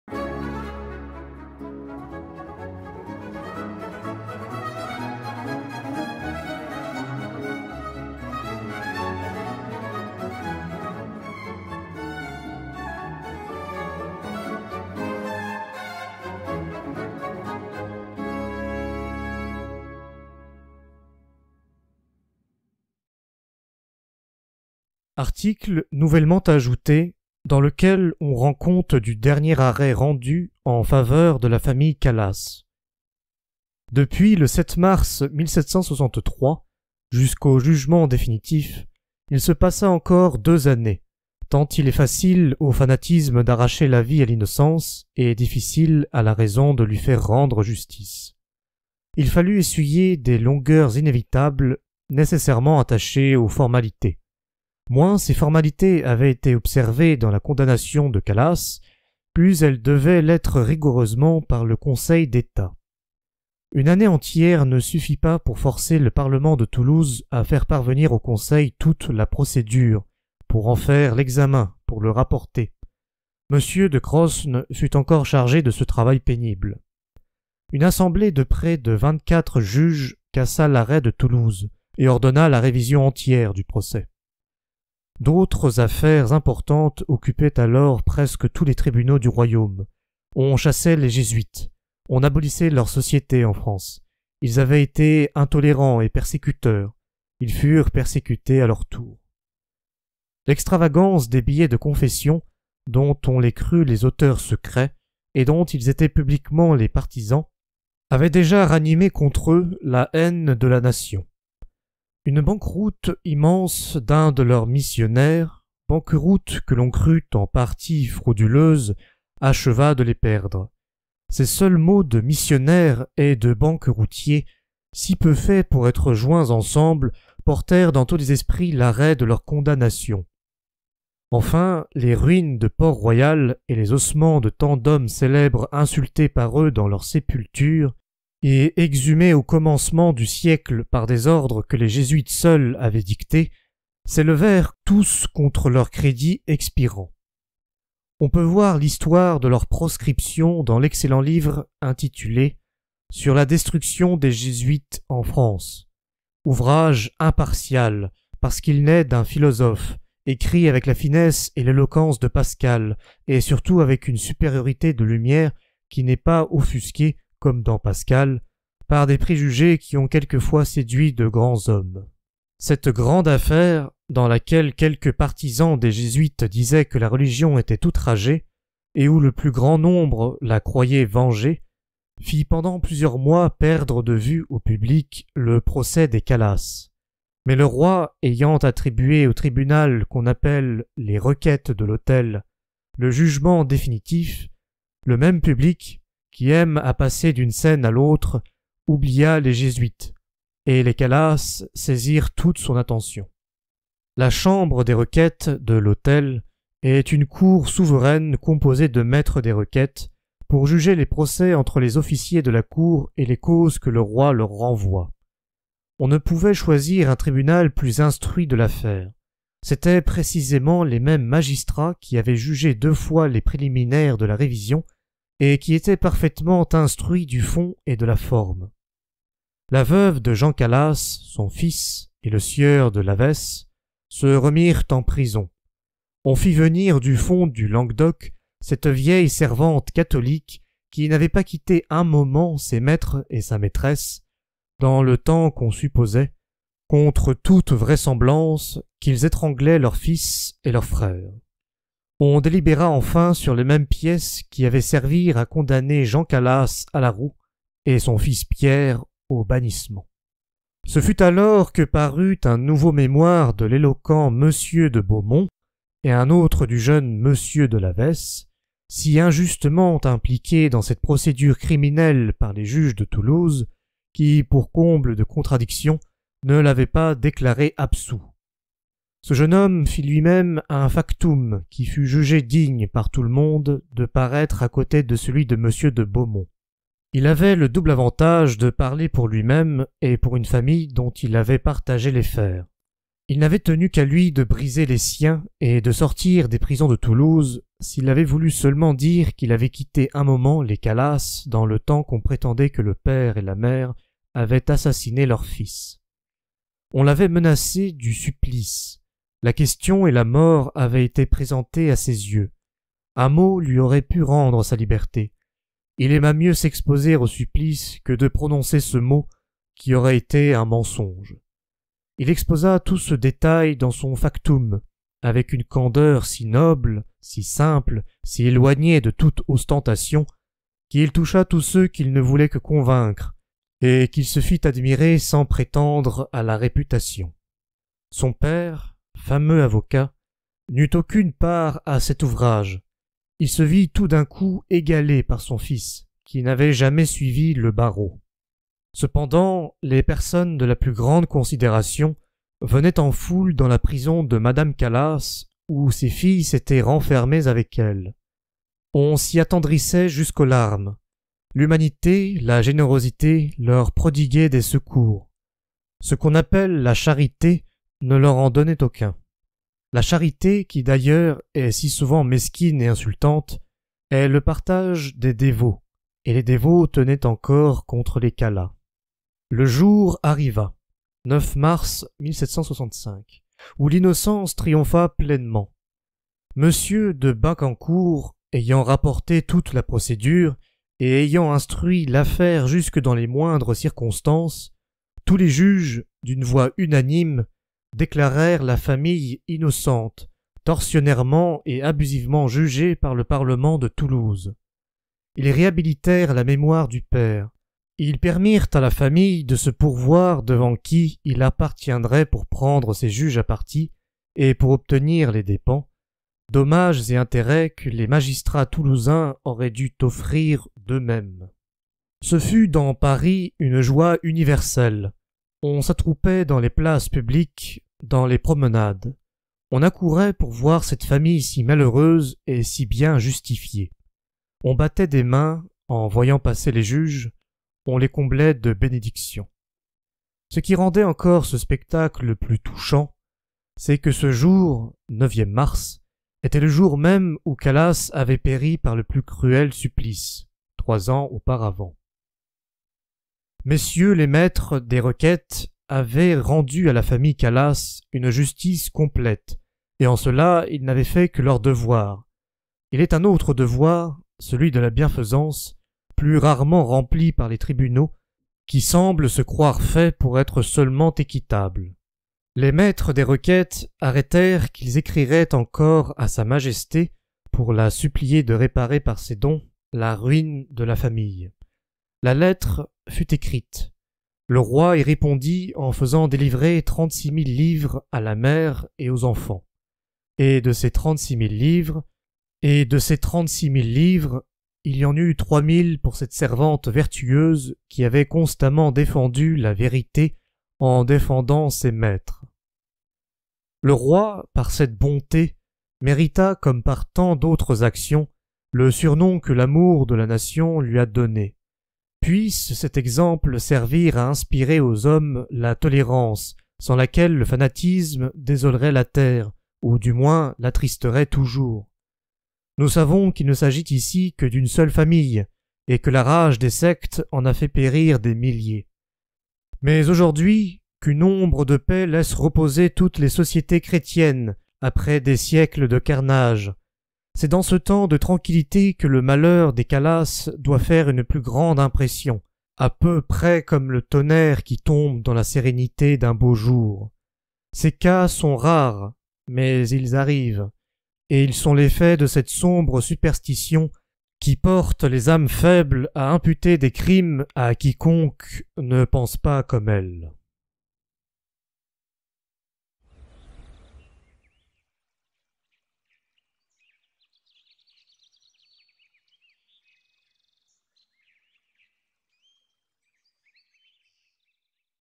"Article nouvellement ajouté [...]" - Traité sur la Tolérance - Voltaire | Livre Audio
📖 : Lecture et interprétation des "Traité sur la Tolérance" de Voltaire par la chaîne du Prosode !

❗  Voici le vingt-sixième chapitre ou article supplémentaire du traité par la chaîne du Prosode ❗

🔸N'hésitez pas à partagez vos impressions dans l'onglet "communauté" de la chaîne et à participer aux différentes opportunités que je vous donne